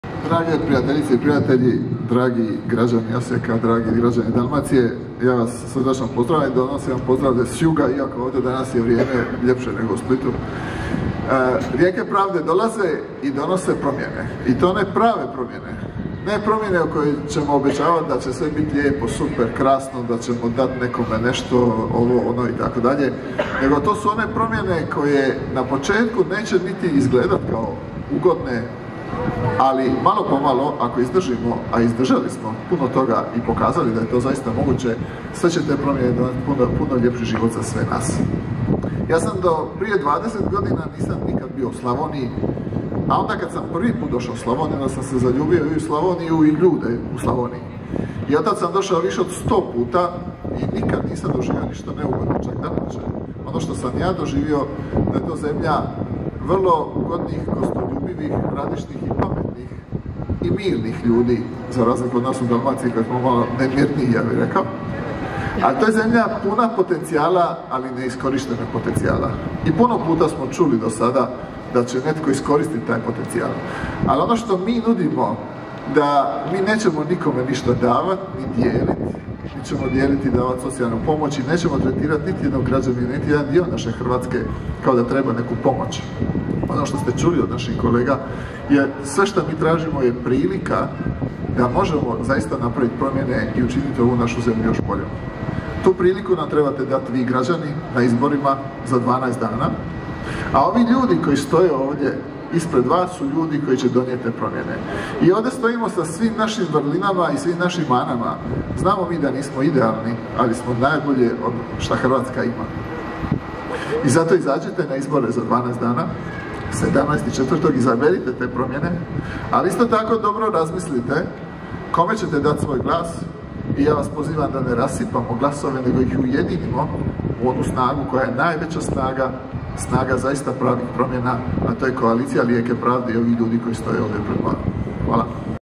Ivica Puljak rekao je da ‘rijeke pravde’ prave promjene, koje na početku neće izgledati kao ugodne
Predstavljanje kandidata i kandidatkinja koalicije Rijeke pravde iz IV. izborne jedinice. Predsjednik SDP-a Peda Grbin bit ce dostupan za izjave nakon predstavljanja.